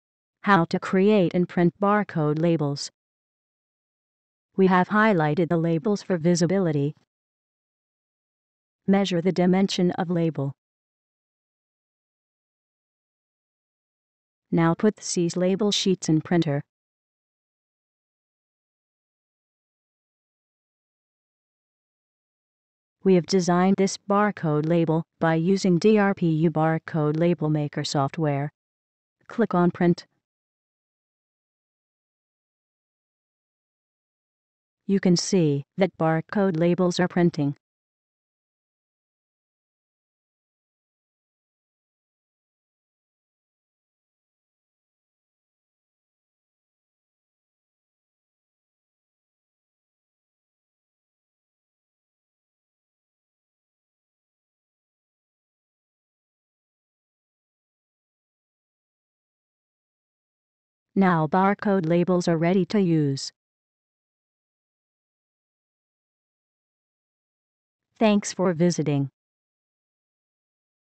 Create and Print Business Barcode Stickers
Now you can easily Create and Print Business Barcode Stickers as per requirement using Barcode Designer Software. Generate barcode labels using different Linear and 2D Barcode symbologies for different industries.